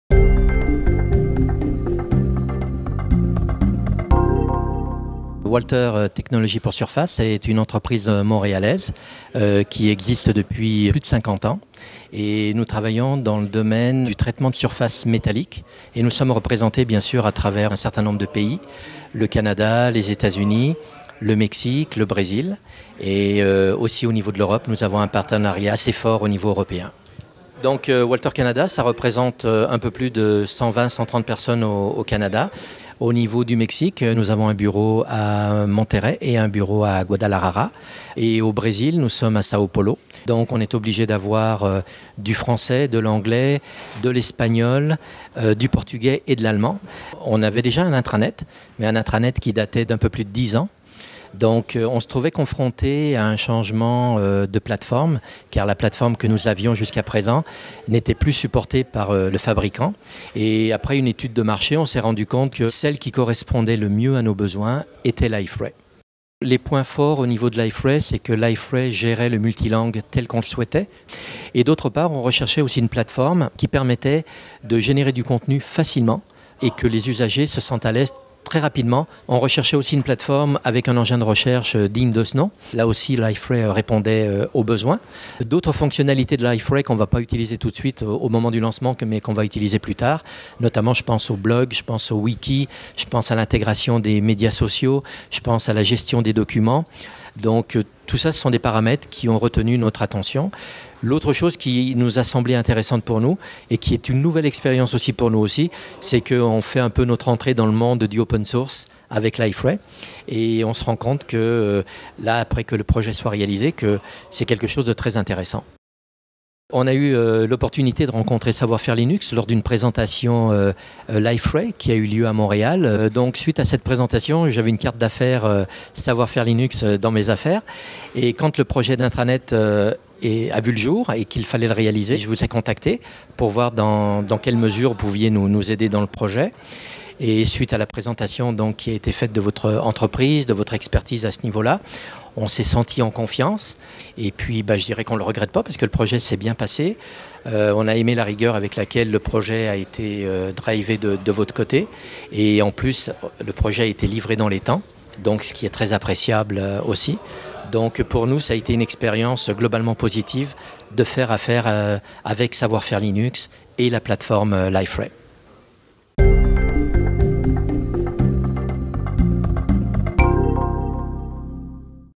Pourquoi faire affaire avec Savoir-faire Linux et la plateforme Liferay ?
Montréal, juin 2013 - Cette courte entrevue de César Strafile, Marketing Communications Manager chez Walter Technologies, explique comment la plateforme Liferay répond aux besoins de communication interne de cette entreprise multinationale et donne ses impressions sur le travail réalisé par Savoir-faire Linux.